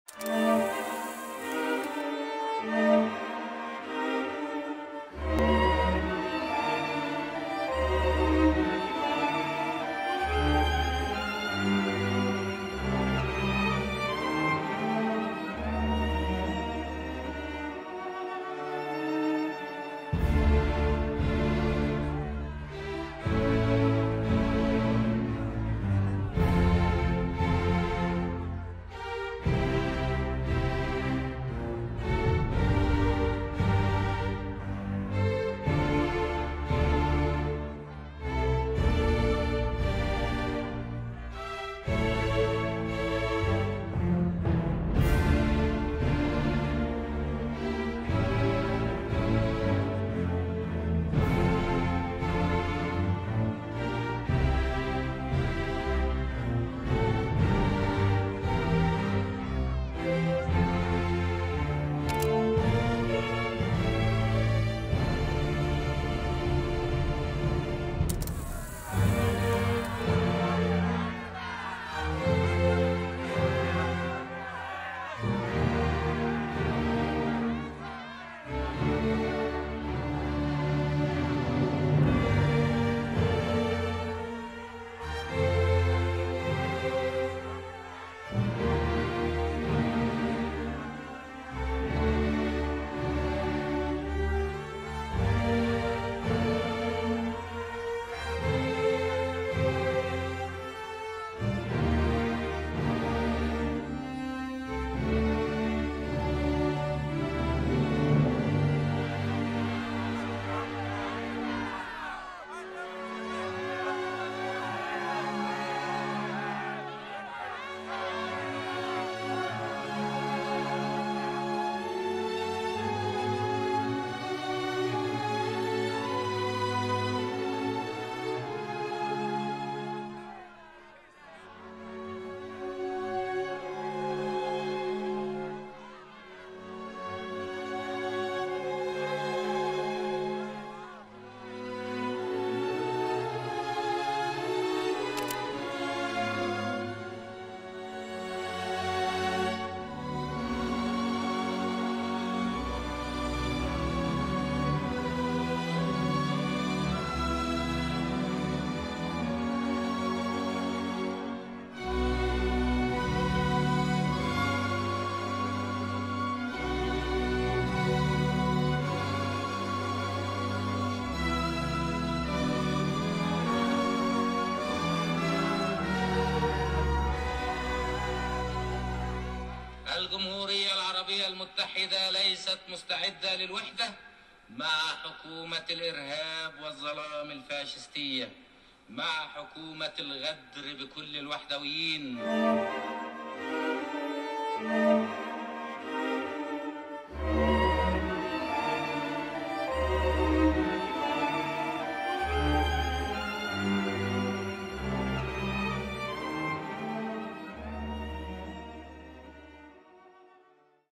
مصر وسوريا والعراق.. حكاية وحدة الـ 100 يوم
حكاية مشروع الوحدة بين مصر وسوريا والعراق والذي مات قبل أن يولد. كيف بدأ وسط الانقلابات وانتهى ببحر من الدماء.